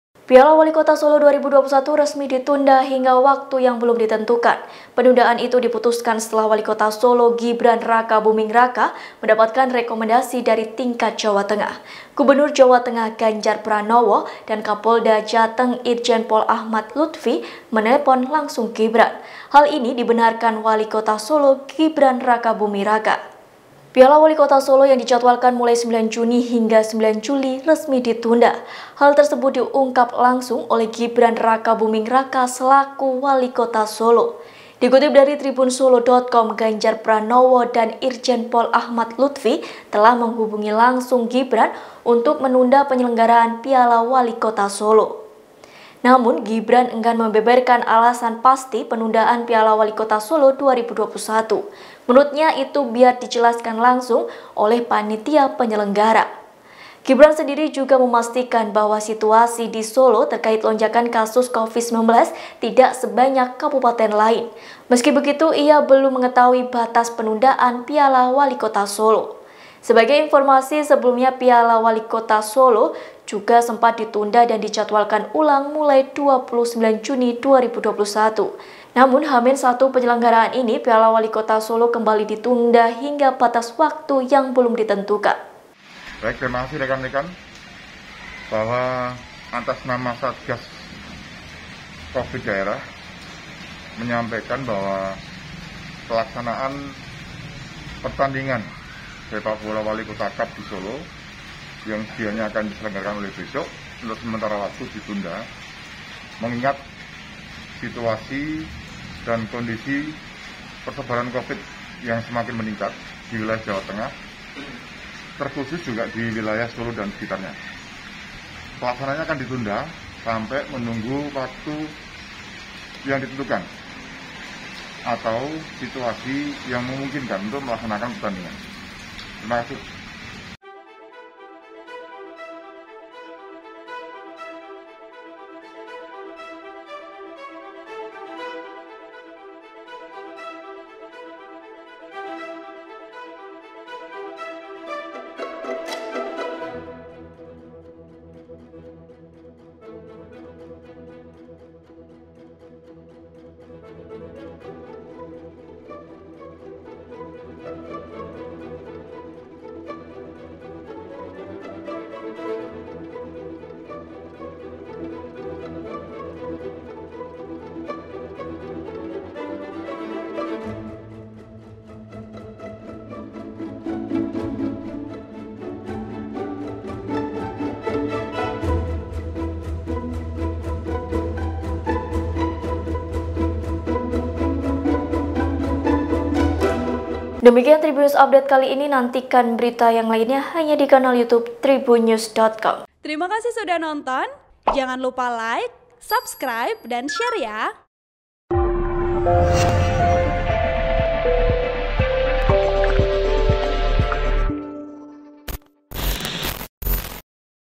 Piala Wali Kota Solo Ditunda hingga Waktu Tak Ditentukan, Gibran Ditelepon Ganjar & Kapolda Jateng
TRIBUN-VIDEO.COM - Piala Wali Kota Solo 2021 resmi ditunda hingga waktu yang belum ditentukan.

Penundaan itu diputuskan setelah Wali Kota Solo, Gibran Rakabuming Raka mendapatkan rekomendasi dari tingkat Jawa Tengah.

Gubernur Jawa Tengah, Ganjar Pranowo dan Kapolda Jateng, Irjen Pol Achmad Luthfi menelpon langsung Gibran.

Hal ini dibenarkan  Wali Kota Solo, Gibran Rakabuming Raka.

"Tadi barusan Kapolda dan Ganjar telepon. Tidak apa-apa. Biar panitia saja yang mengumumkan," kata Gibran, Senin (28/6/2021).

Piala Wali Kota Solo yang dijadwalkan mulai 9 Juni hingga 9 Juli resmi ditunda.

Hal tersebut diungkapkan langsung oleh Gibran Rakabuming Raka selaku Wali Kota Solo.

Dikutip dari TribunSolo.com, Ganjar Pranowo dan Irjen Pol Achmad Luthfi telah menghubungi langsung Gibran untuk menunda penyelenggaraan Piala Wali Kota Solo.

"Tadi barusan Kapolda dan Ganjar telepon. Tidak apa-apa. Biar panitia saja yang mengumumkan," kata Gibran kepada Tribun Solo, Senin (28/6/2021).

Namun, Gibran enggan membeberkan alasan pasti penundaan Piala Wali Kota Solo 2021.

Menurutnya, itu biar dijelaskan langsung panitia penyelenggara.

"Tunggu nanti malam," ucapnya.

Gibran sendiri juga memastikan bahwa situasi di Solo terkait lonjakan kasus Covid-19 tidak sebanyak kabupaten lain.

Meskipun begitu, ia belum mengetahui batas penundaan Piala Wali Kota Solo.

"Di Solo kita tidak separah kabupaten lain. Untuk alasan-alasan pastinya biar panitia saja," ucapnya.

"Belum Tahu," ujarnya.

Sebagai informasi, sebelumnya Piala Wali Kota Solo juga sempat ditunda dan dijadwalkan ulang mulai 29 Juni 2021.

Namun H-1 penyelenggaraan ini Piala Wali Kota Solo kembali ditunda hingga batas waktu yang belum ditentukan.

"Belum Tahu," ujarnya.(Tribun-video.com/ TribunSolo.com)

Artikel ini telah tayang di TribunSolo.com dengan judul BREAKING NEWS: Piala Wali Kota Solo Ditunda, Gibran Dapat Telepon dari Ganjar & Kapolda Jateng,